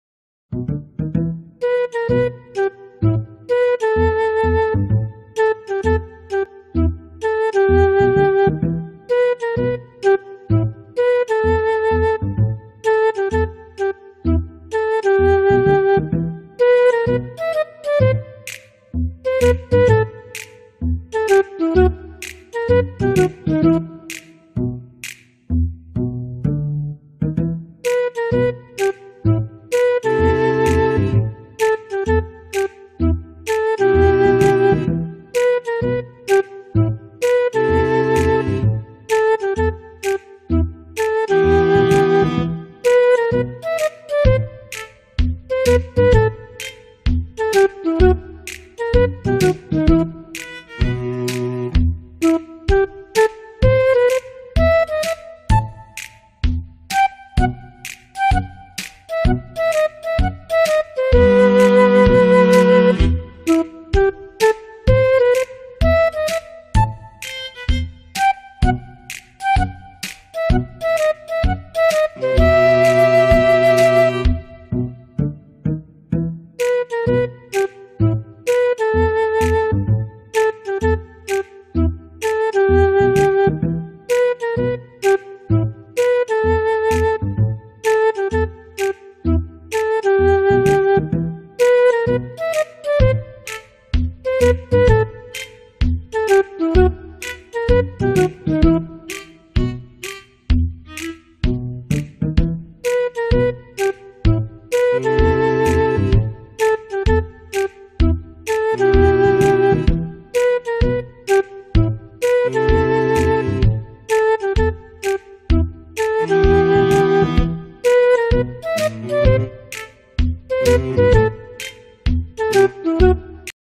BULE DIBALI BIKIN NGAKAK
kejadian lucu bule dibali.. hanya sebatas hiburan saja.